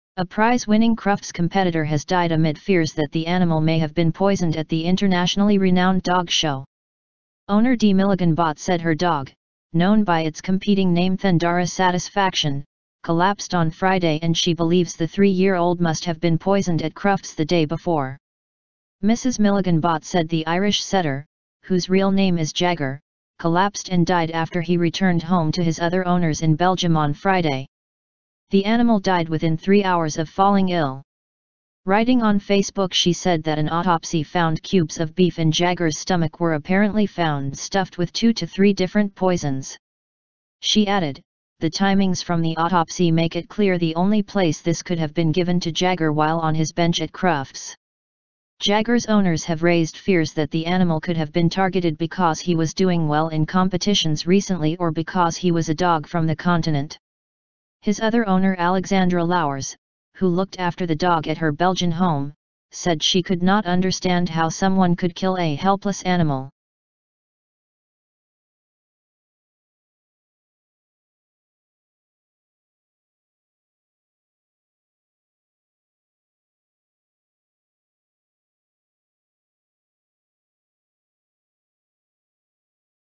Prize-winning red setter is 'murdered' at Crufts: Police called in
Prize-winning red setter is 'murdered' at Crufts: Police called in after Jagger's owner claims he was fed poisoned cubes of beef